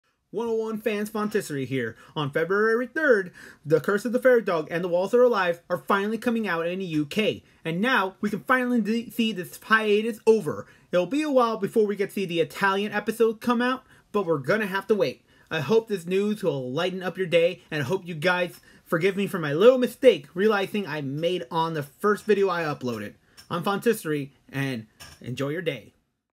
101 Dalmatian Street - UK news update
101 fans the UK it’s finally releasing episodes, the fairy dog and the walls are alive, but will have to wait until more news to comes out for the other episode to air in the UK.